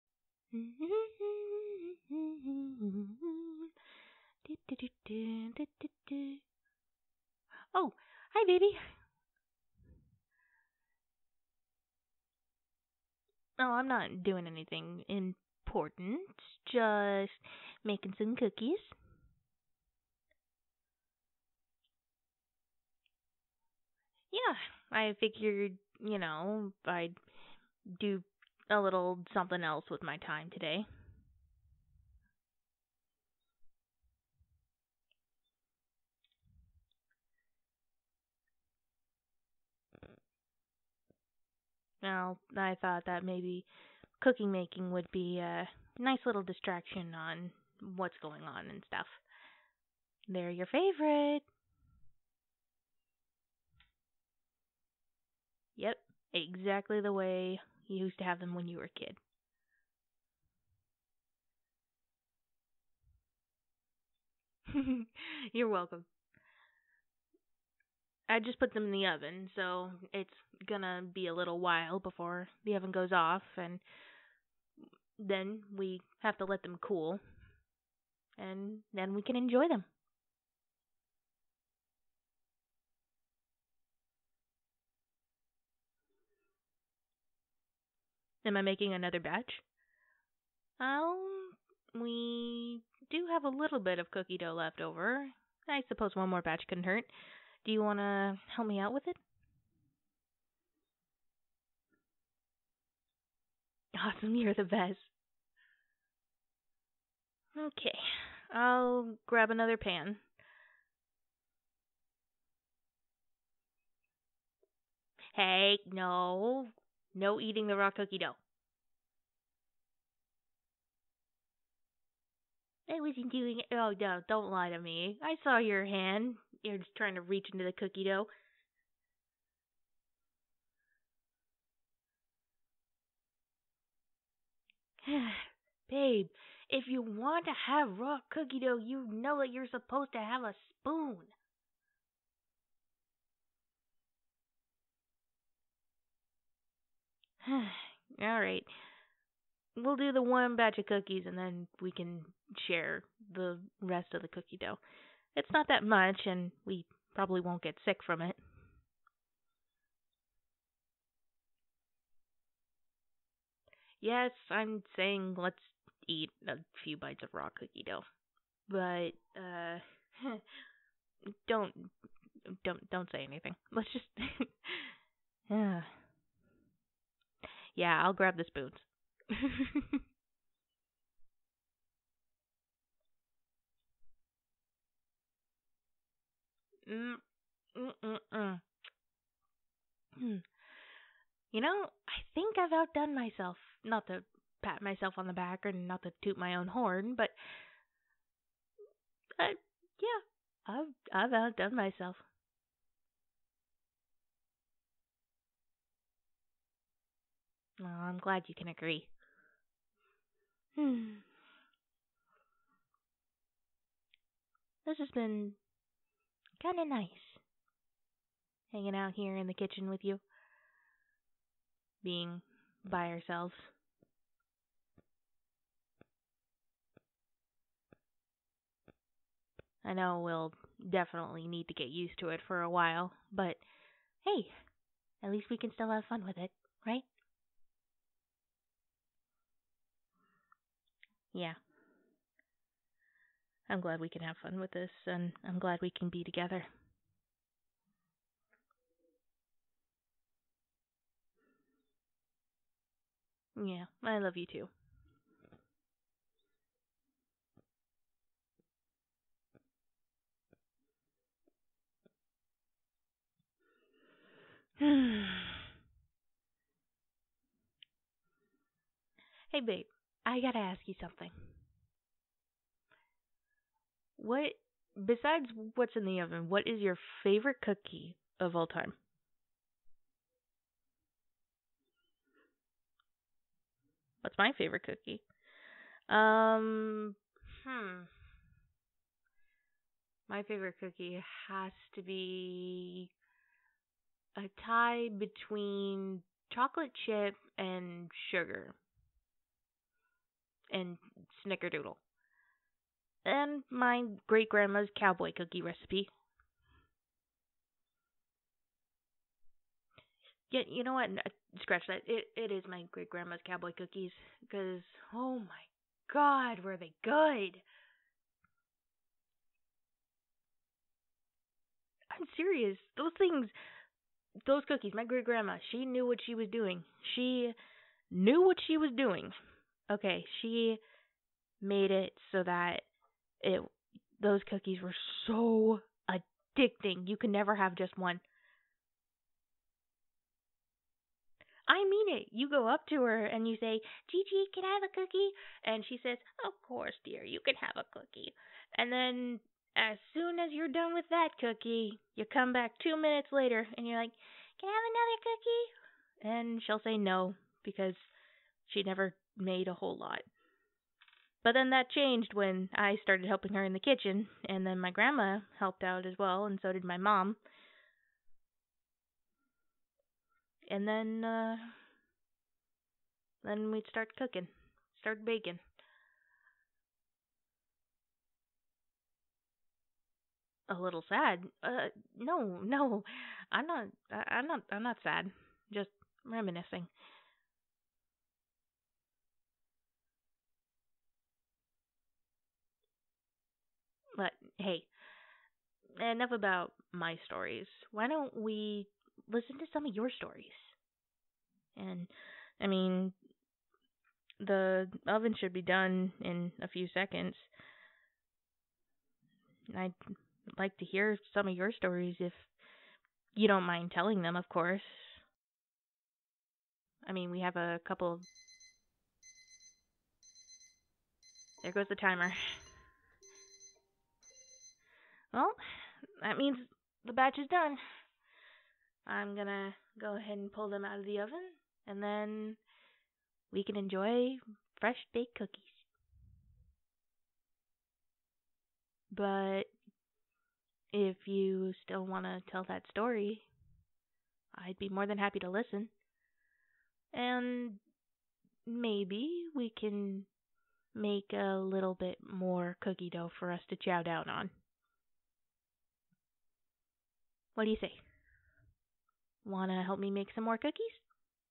Cookie Time(Girlfriend Roleplay)(ASMR)(Story Time)
To help kill our boredom I thought maybe I could make some of your favorite cookies, and maybe share a story or tow together

If you are wanting to see something different for artwork, click this link here (must be over 18):